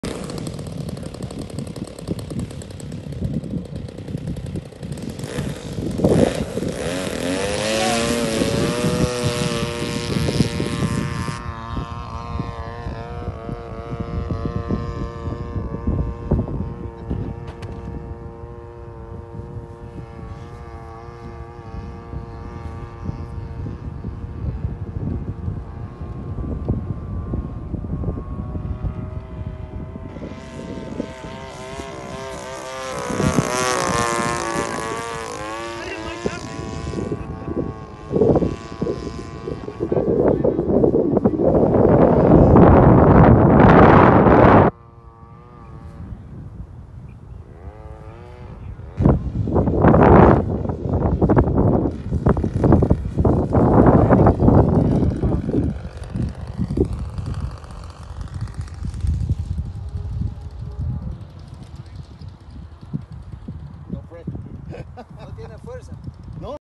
Despegando y aterrizando con viento Rc Airplane
Día con viento 18 millas
Super cub